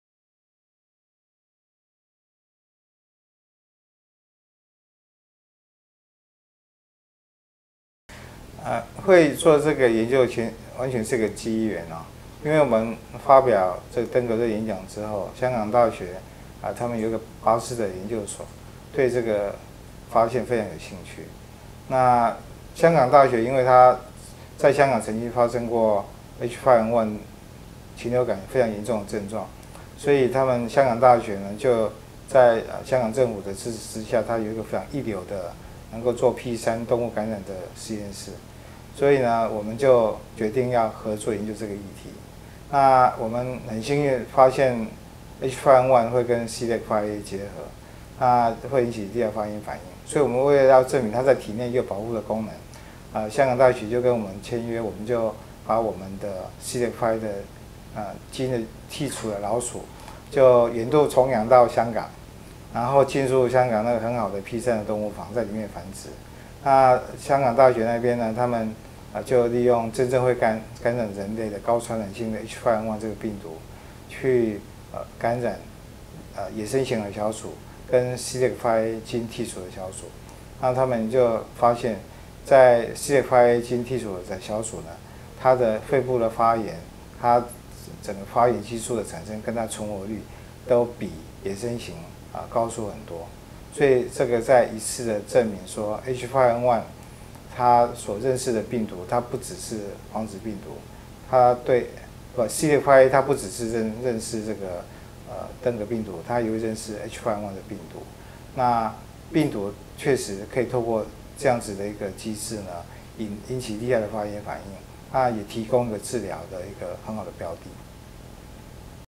2. 與香港大學巴斯德研究中心合作，證實剔除CLEC5A基因的小鼠，對於H5N1流感反應相對輕微許多
Turn English Sub on, if necessary. (bottom right corner of timeline)